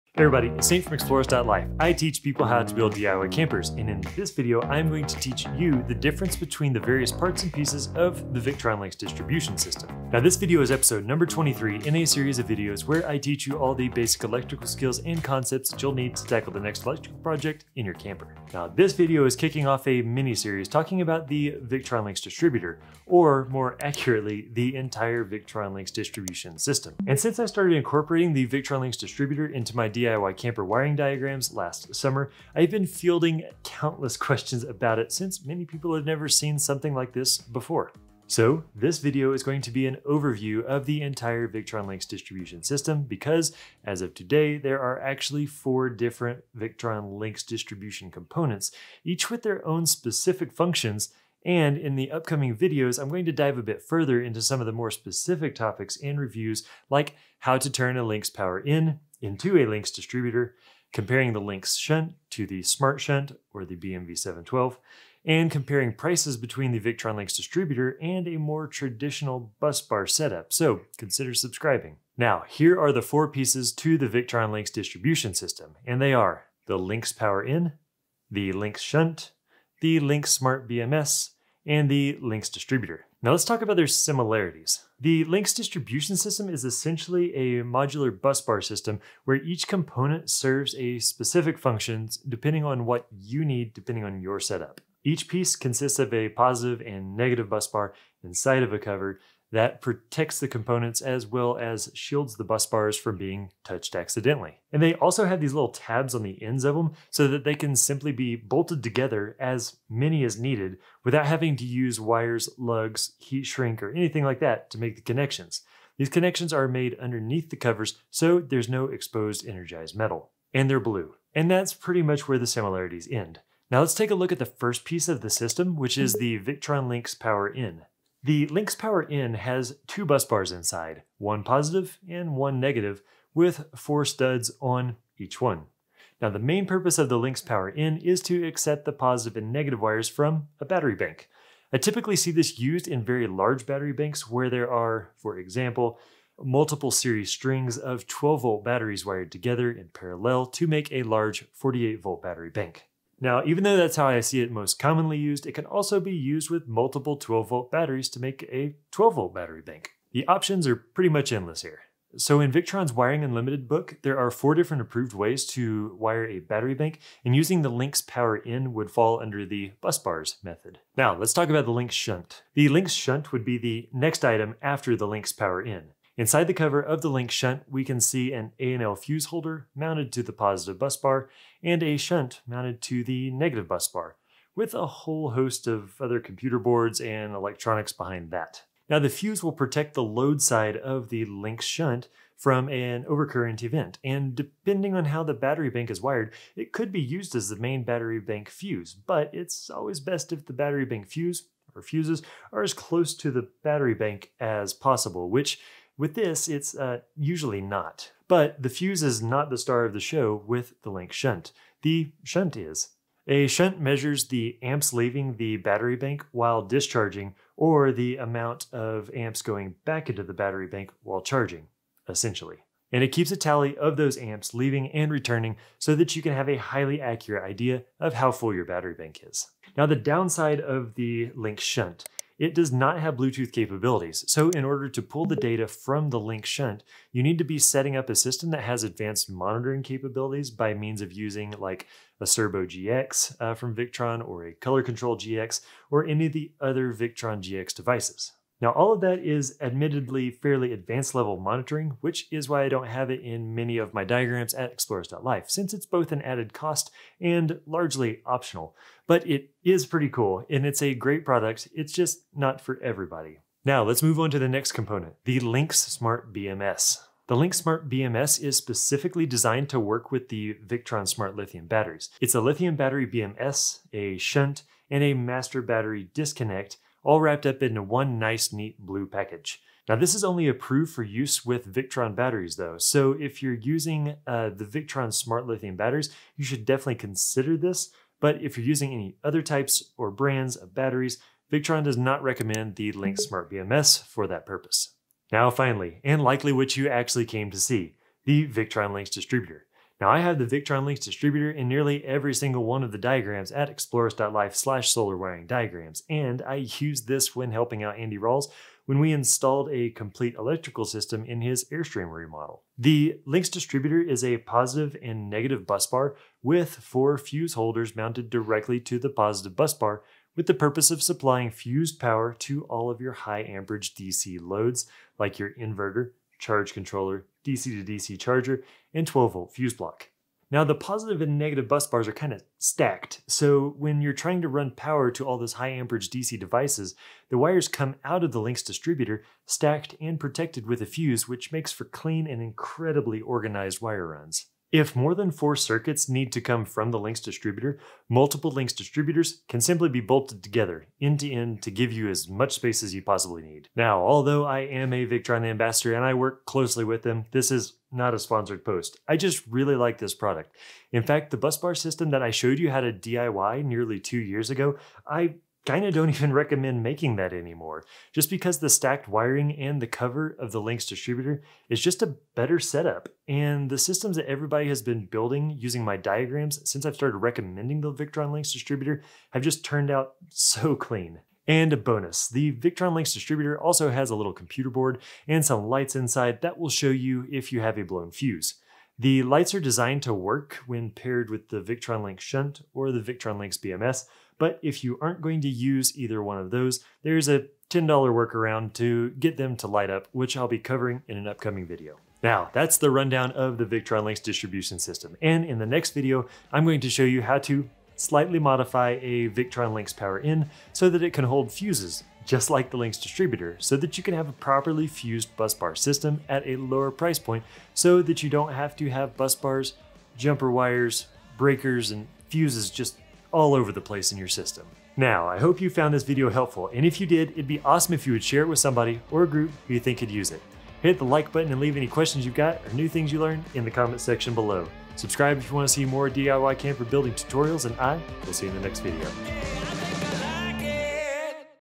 Victron Lynx Distribution System Review for a DIY Camper Electrical System
The Victron Lynx Distribution system is a modular busbar system where each component serves a specific purpose.  This video will teach you the overarching purpose of each of the pieces.

Victron Lynx Power-In
Victron Lynx Shunt
Victron Lynx Distributor

➤Need additional & personalized help?  Consider joining my private group